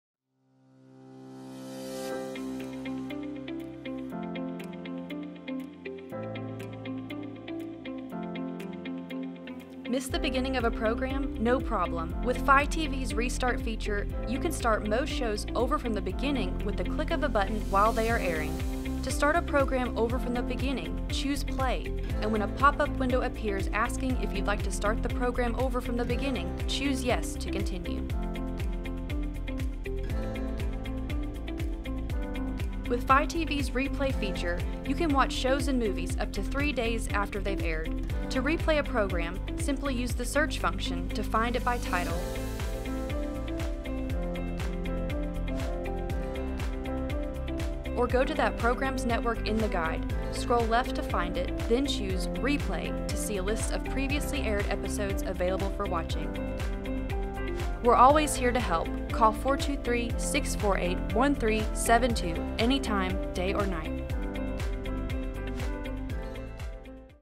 How to Use Restart Replay on EPB Fi TV
Learn how to Restart live television from the beginning and Replay shows and movies up to three days after they air on EPB Fiber Optics Fi TV with helpful, step-by-step instructions.